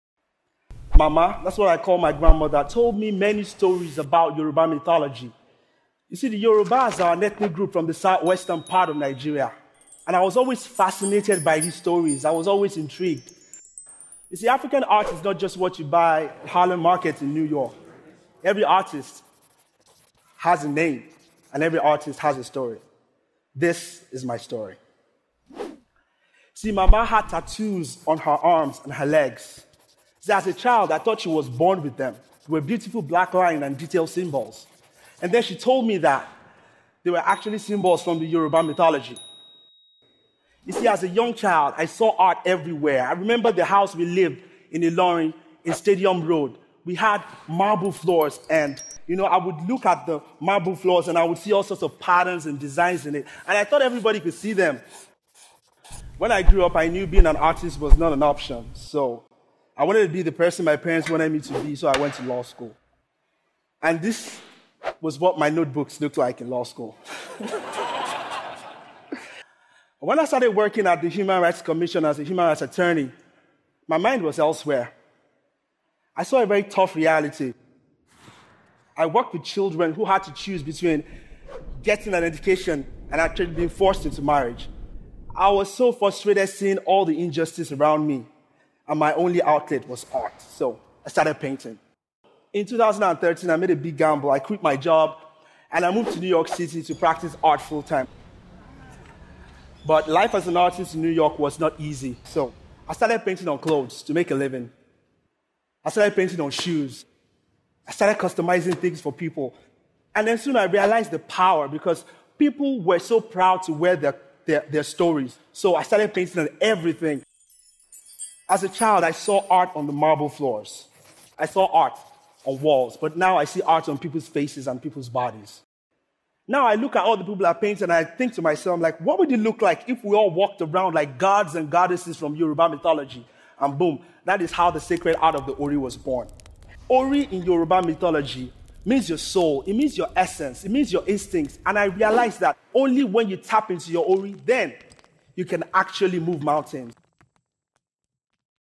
The Sacred Art of the Ori - Short Animation
Originally a project for class. Animation of Laolu Sebanjo's Ted Talk.